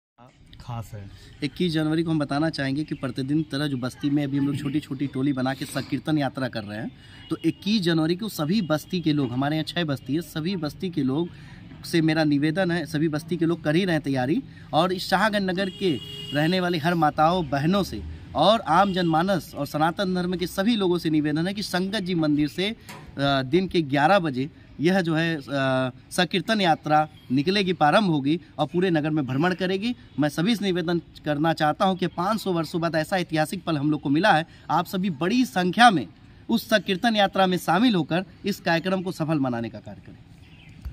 रामलला की प्राण प्रतिष्ठा को लेकर राममय हुआ शाहगंज, कार्यक्रमों की तैयारियां जोरों पर धीरज पाटिल
रामलला की प्राण प्रतिष्ठा को लेकर राममय हुआ शाहगंज, कार्यक्रमों की तैयारियां जोरों पर

जौनपुर जिला अंतर्गत नगर और ग्रामीण इलाके रामलला की प्राण प्रतिष्ठा को लेकर राममय हो गये हैं । अयोध्या में श्री राम के नवनिर्मित मंदिर में प्राण प्रतिष्ठा के अवसर पर कई कार्यक्रमों की तैयारी हो रही है ।

आप को बता दे कि जौनपुर जिला के शाहगंज नगर में श्रीराम के स्वागत की तैयारी जोर-शोर से हो रही है। यहां के लोगों में अयोध्या में होने वाले श्रीराम मंदिर प्राण प्रतिष्ठा को लेकर उत्साह चरम पर है । गांव से लेकर शहर तक राम नाम की बयार बह रही है । 22 जनवरी तक यहां पर कई तरह के कार्यक्रमों के आयोजन किए जा रहे हैं। 22 जनवरी को अयोध्या श्रीराम मंदिर प्राण प्रतिष्ठा का सीधा प्रसारण, प्रसाद वितरण, भंडारा आदि का आयोजन कई मंदिरों में किया गया है।

21जनवरी को एक भव्य शोभायात्रा भी निकाली जायेगी, जिसमें हजारों लोग भजन गाते हुए नगर भ्रमण के लिए निकलेंगे । आयोजक कहते हैं कि पूरी नगरी को राममय बनाने की तैयारी है। आयोजन समिति  ने कहा कि हम सभी के लिए 22 जनवरी का वो पल अद्भुत होगा जब भगवान अयोध्या में पधारेंगे।

जौनपुर से दिवाकर मिश्रा के साथ दीपक सिंह की रिपोर्ट